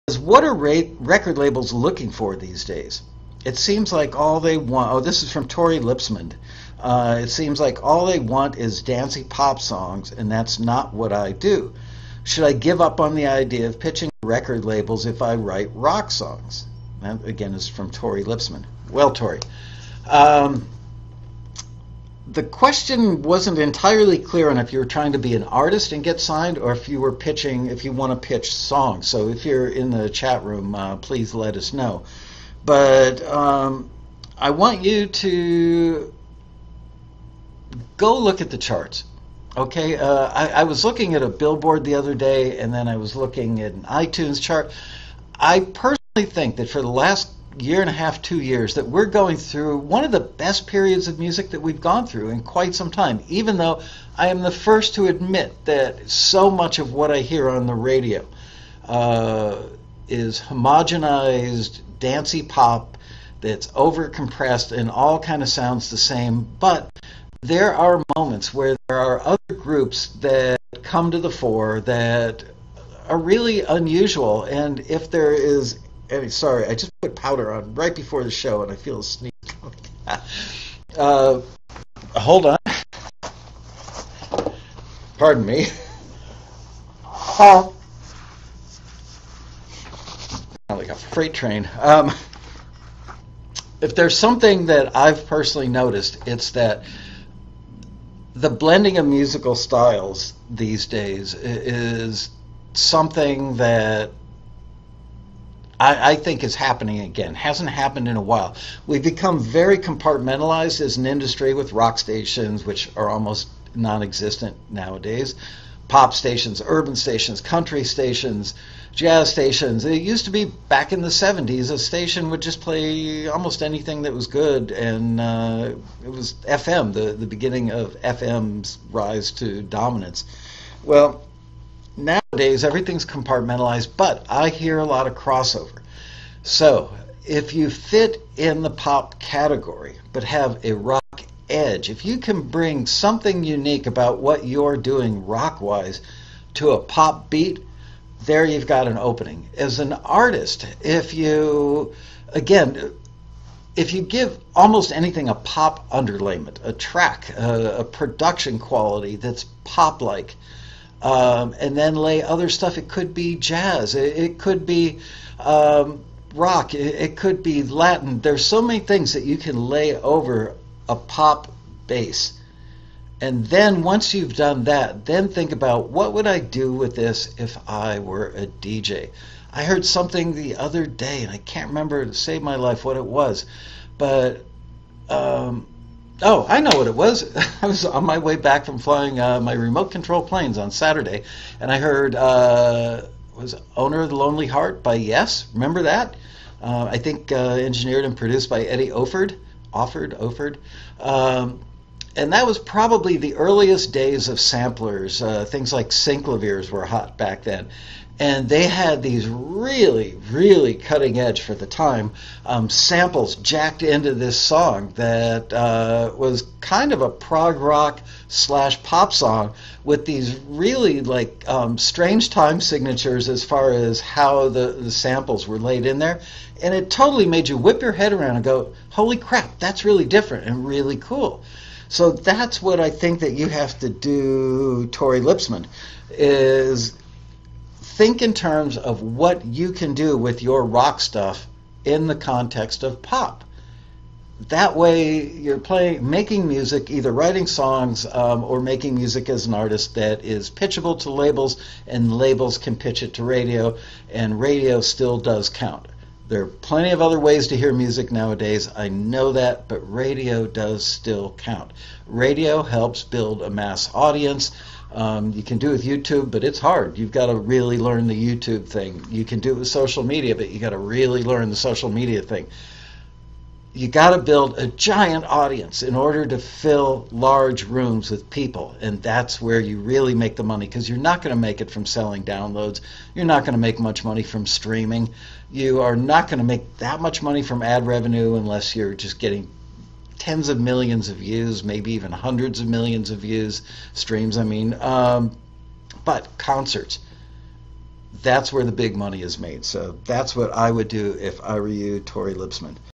What are RECORD LABELS Really LOOKING FOR?
In this short clip of TAXI TV, Michael (TAXI Music CEO) gives you the inside scoop on what record labels are really looking for when they sign an artist.

Rub Elbows with Music Industry Pros at Our FREE Yearly Convention